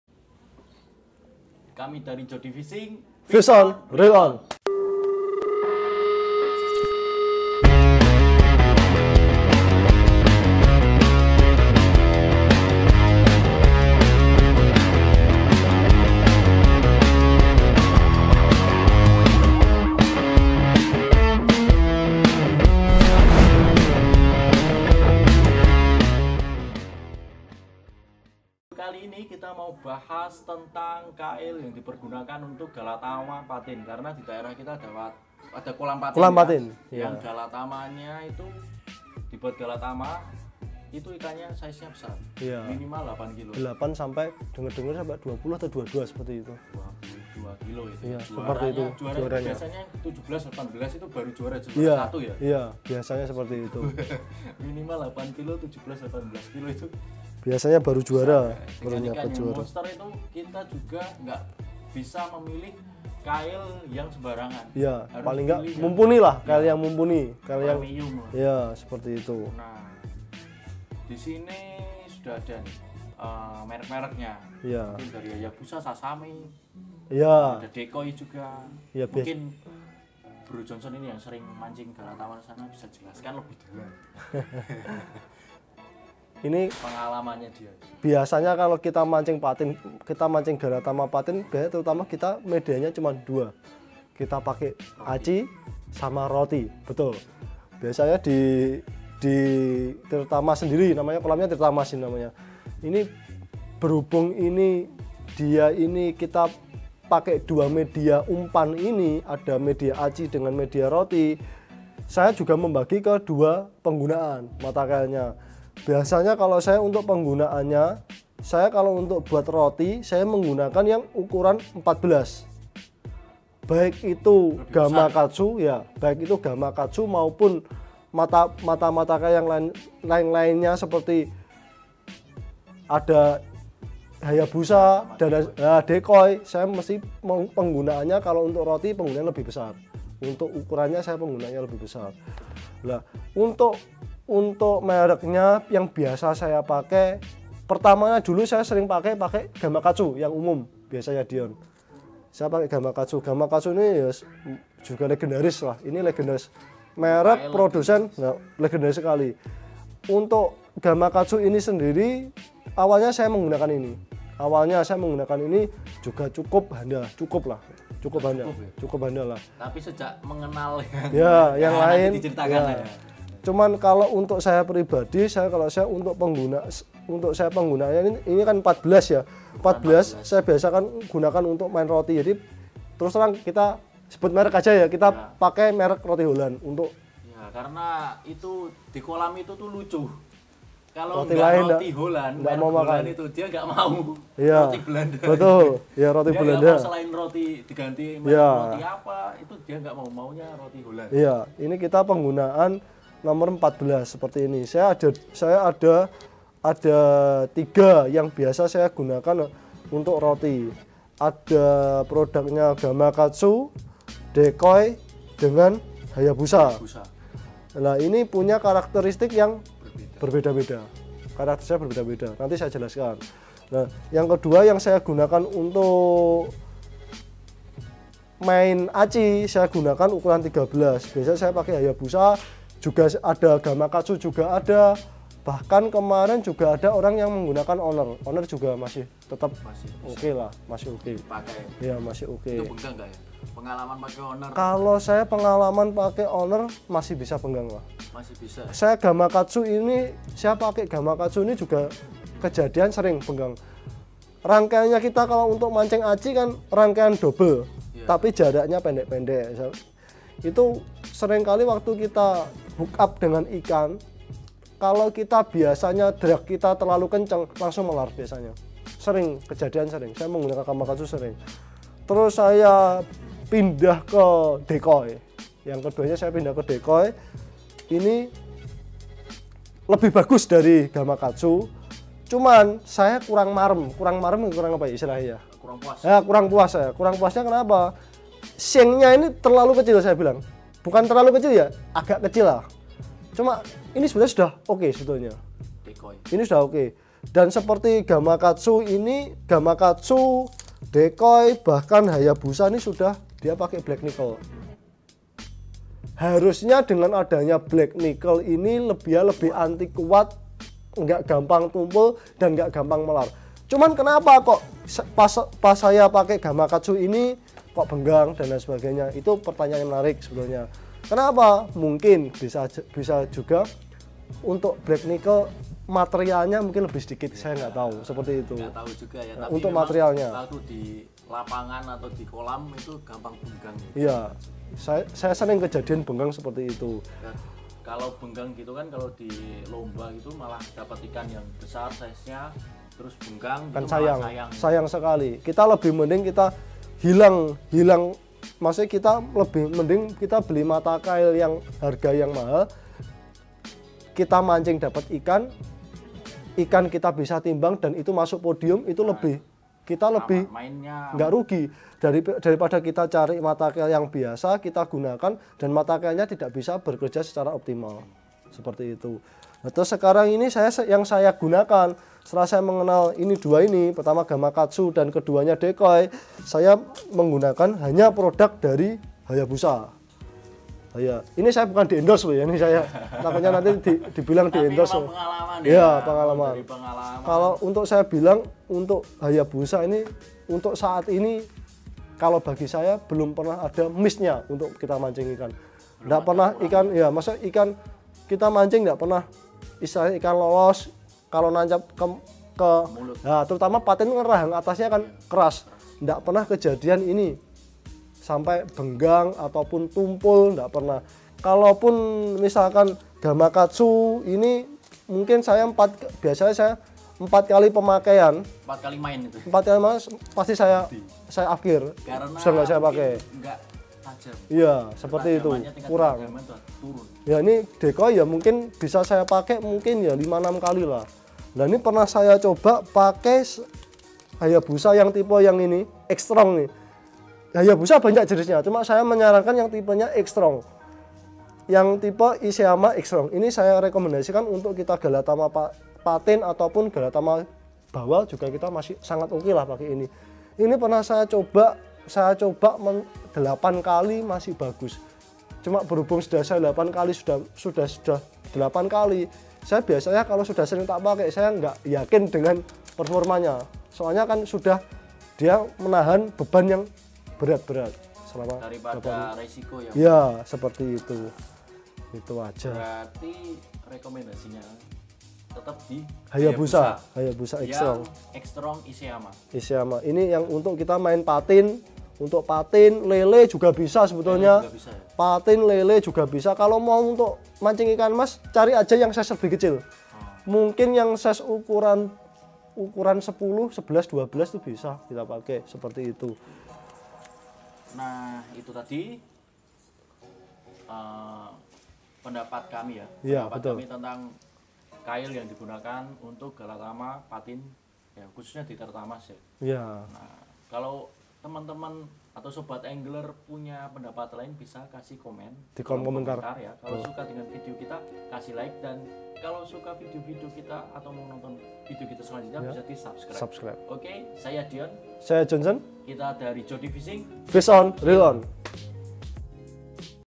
Tips Pemilihan & Pengenalan Kail (Hook) Premium
Mata kail rekomendasi yang kami berikan untuk para angler mania yang bisa di gunakan untuk lomba dan galatama,. Video in hanyalah pendapat kami, jika angler mania memiliki pendapat lain dan pengalaman menggunakan mata kail lainya bisa diskusi lewat kolom komentar kami, terimaksih sudah menonton, FISH ON REEL ON !!